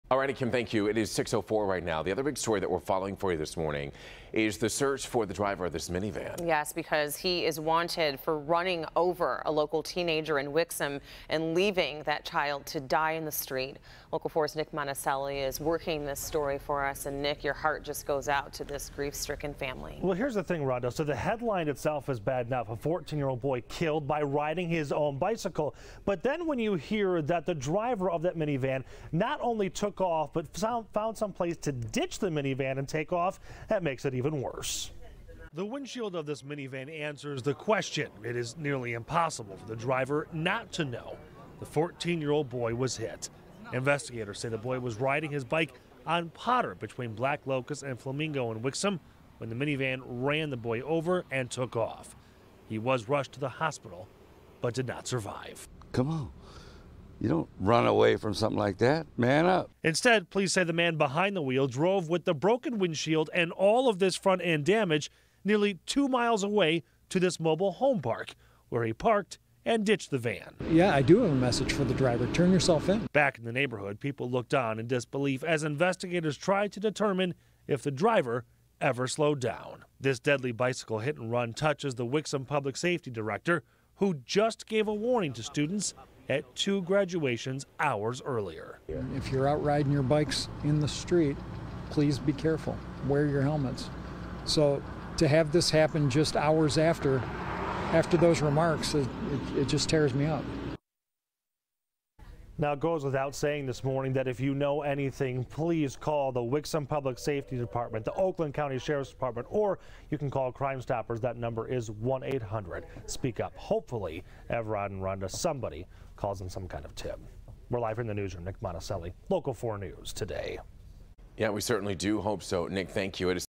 14-year-old killed in hit-and-run on Potter Road in Wixom
Wixom police are working to find the driver of a minivan.